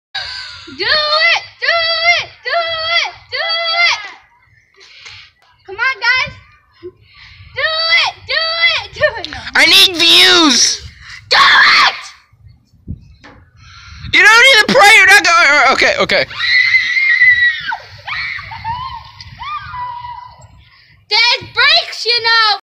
Alien rides bike down hill
I was visiting my grandpa's house with my sis and we did some stupid stuff